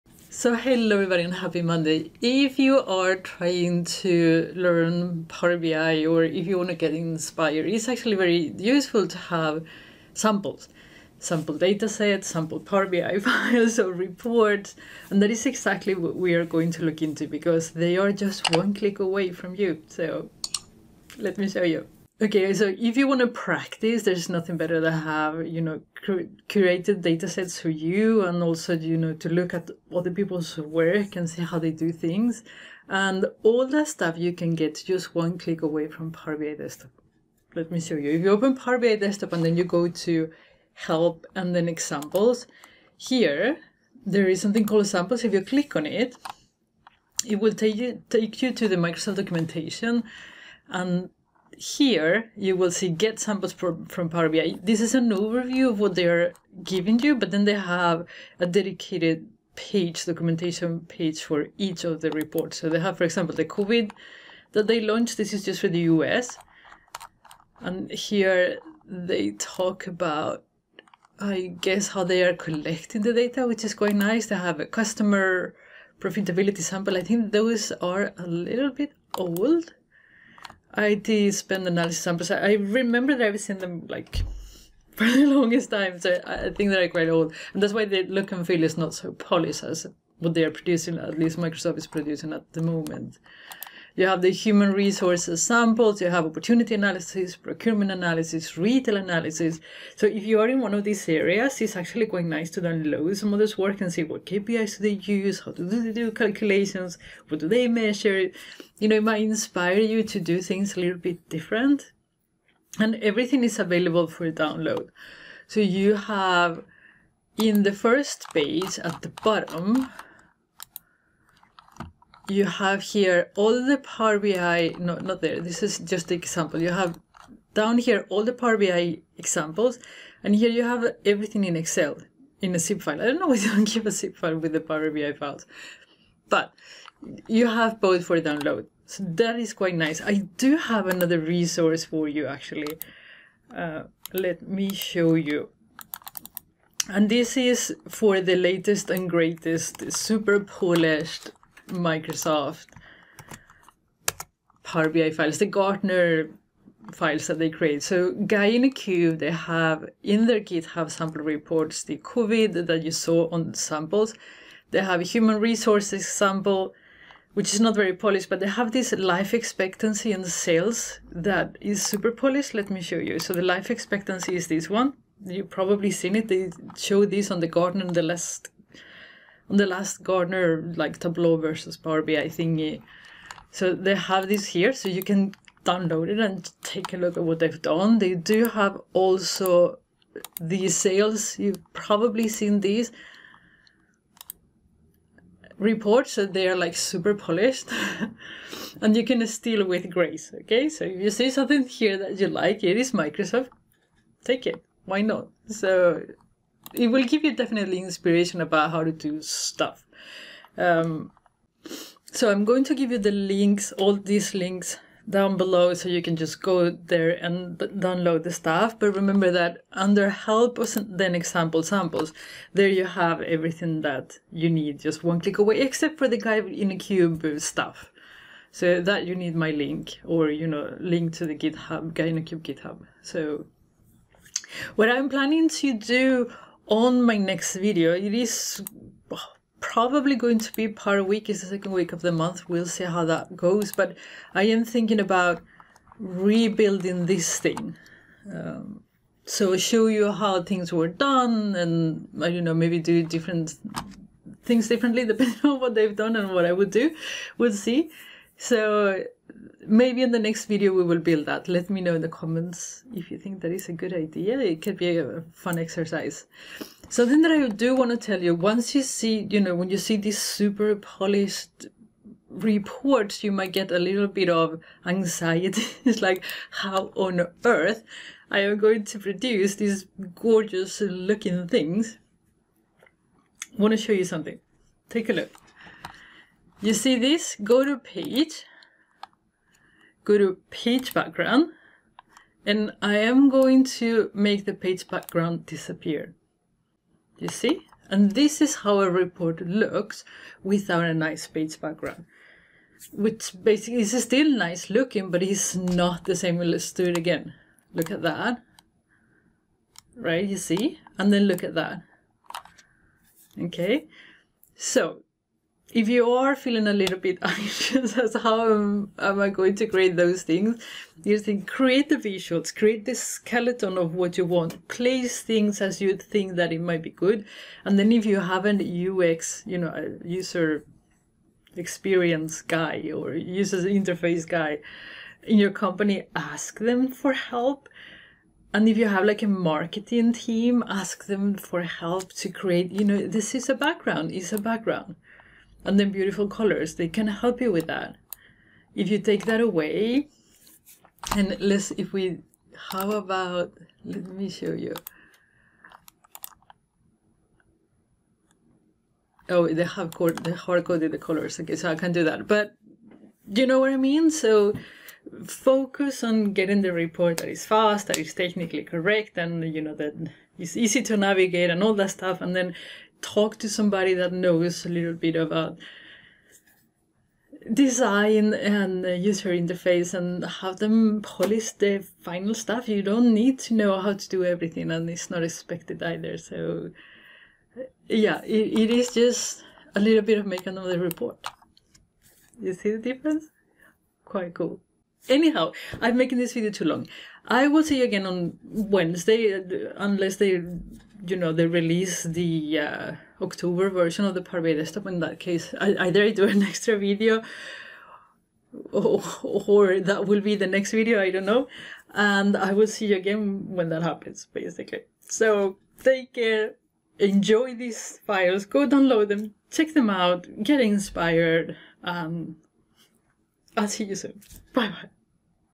Download sample datasets and reports for Power BI and Excel
Sample datasets and reports are great for learning and getting inspiration so in todays video I will share some resources you can download and use right away!